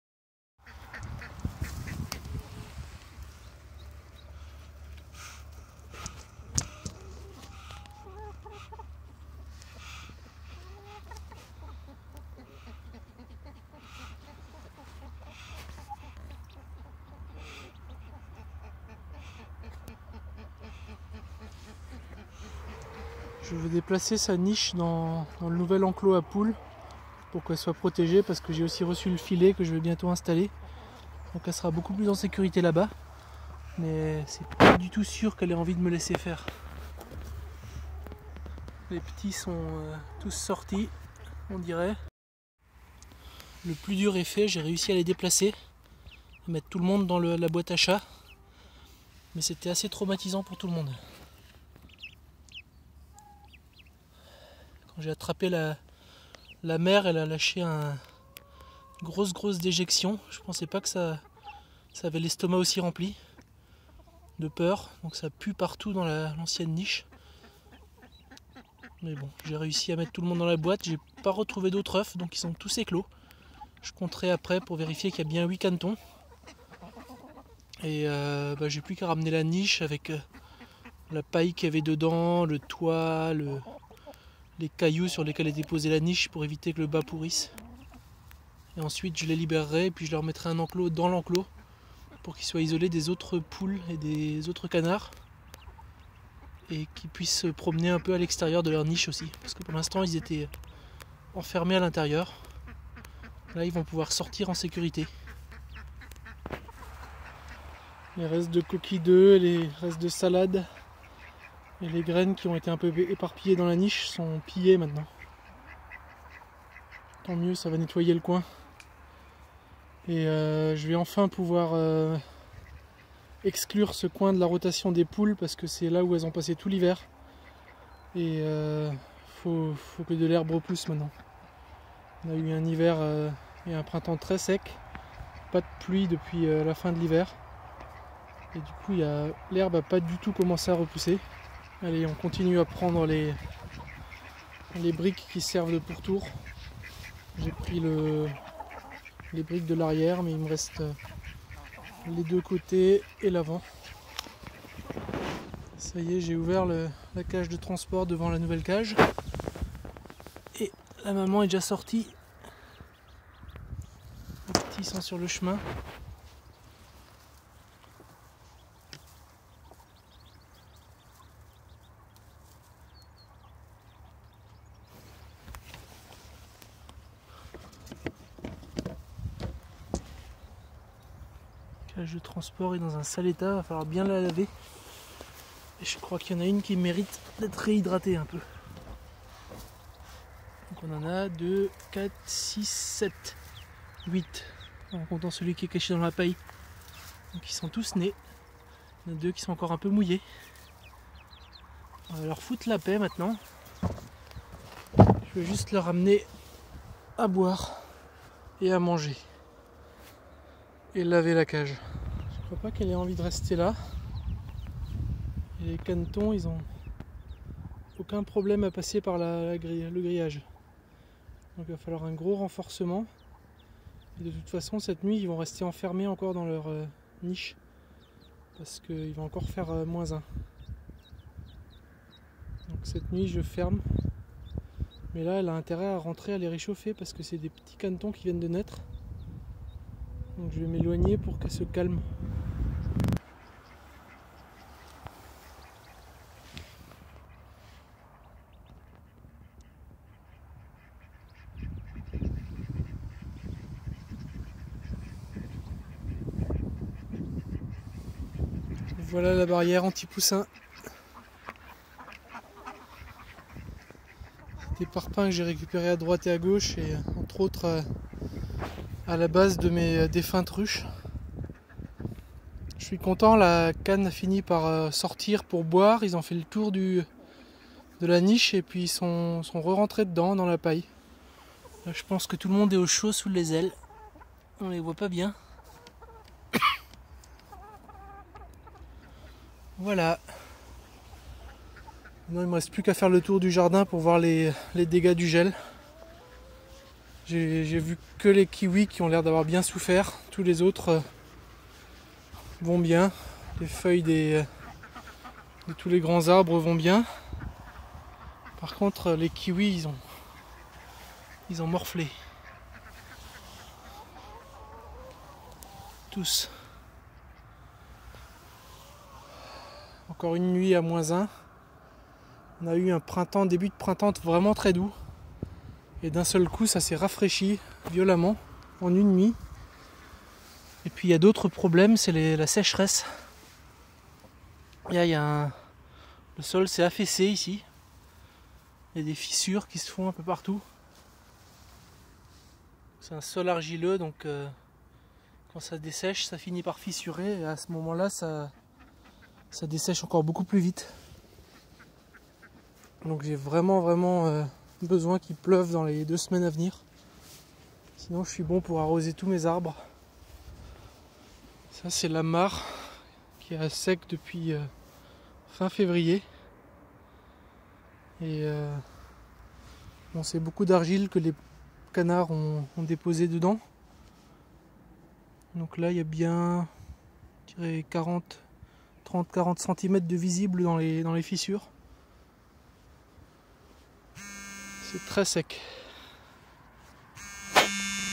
Élevage de canetons - S01E17 - Relocalisation de la cane et de ses 8 canetons nouveaux-nés
Tous les canetons ont maintenant éclos sous la cane de barbarie. 8 canetons pour 8 œufs. Ça fonctionne très bien, la couvaison naturelle (pour rappel, avec la première couvaison artificielle j'ai eu 9 canetons/12 œufs).

Le moment est venu de les mettre en sécurité derrière deux clôtures, pour qu'ils puissent sortir de leur niche sans interférer avec les autres volailles.